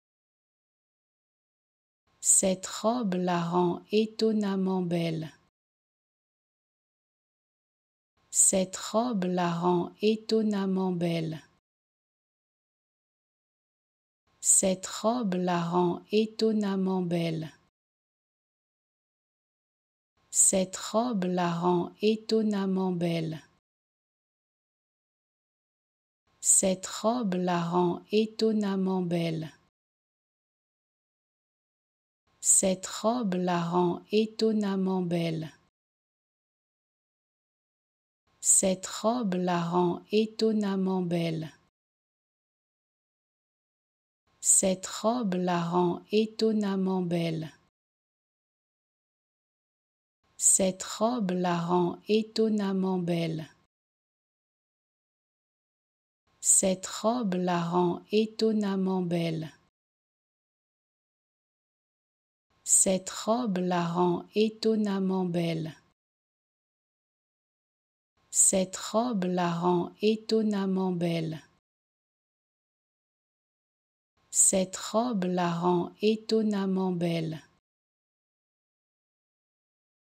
How to Pronounce ''Cette robe la rend étonnamment belle'' Correctly in French
Learn how to say and properly pronounce ''Cette robe la rend étonnamment belle'' in French with this free pronunciation tutorial. Apprenez à prononcer des mots en français grâce à nos tutoriels.